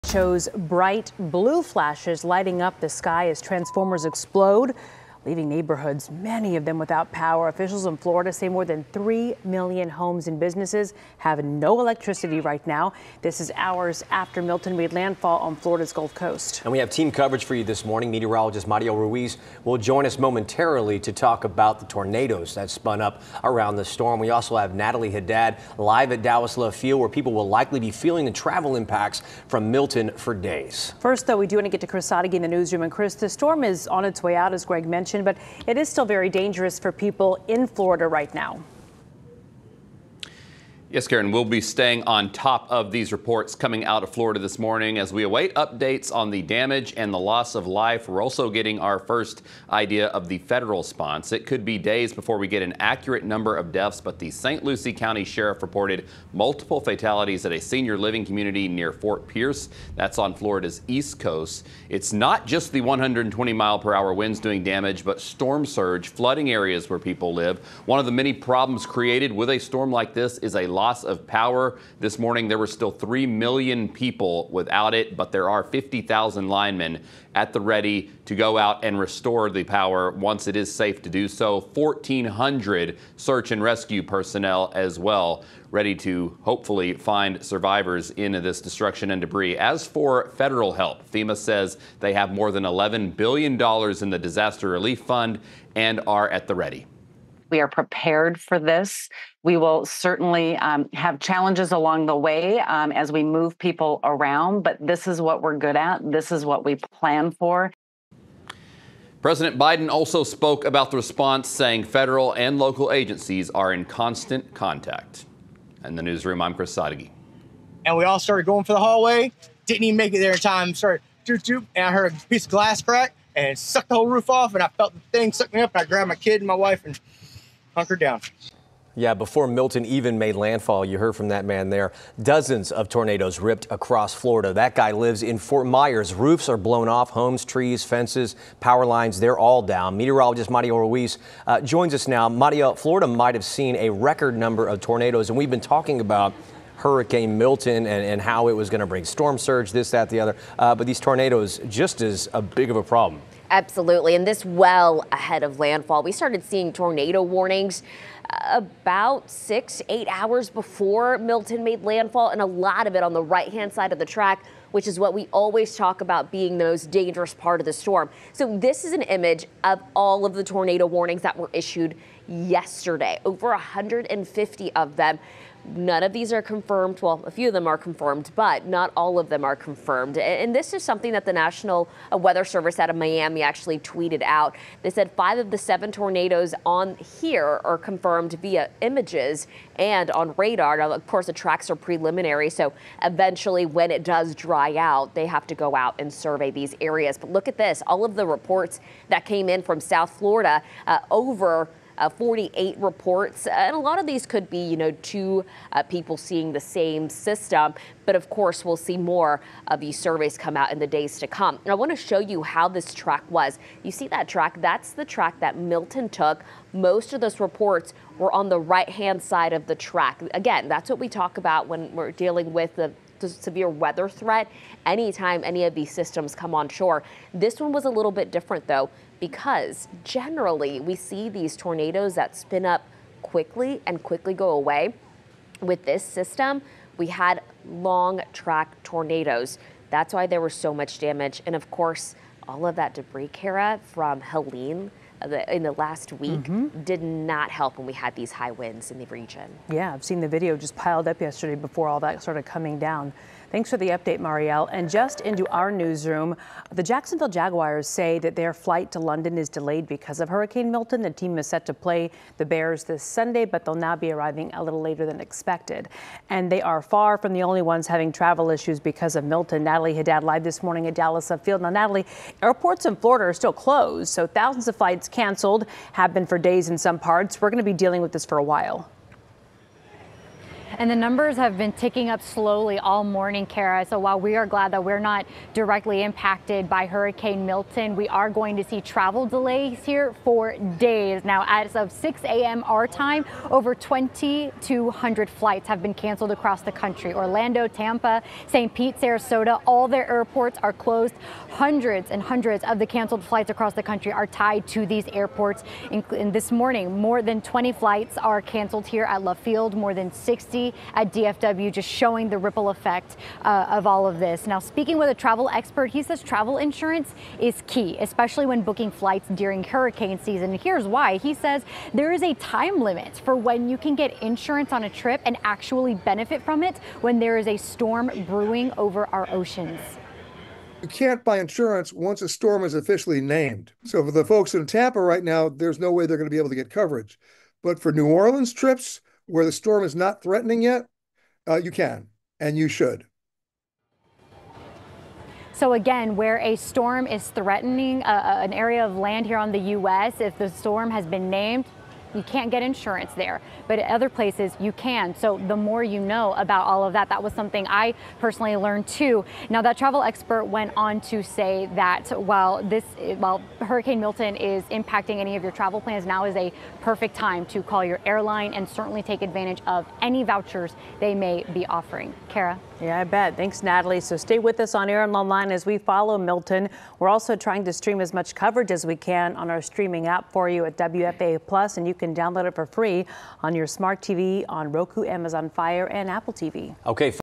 Hurricane Milton aftermath: Latest damage updates, track, travel impacts
Hurricane Milton made landfall Wednesday night.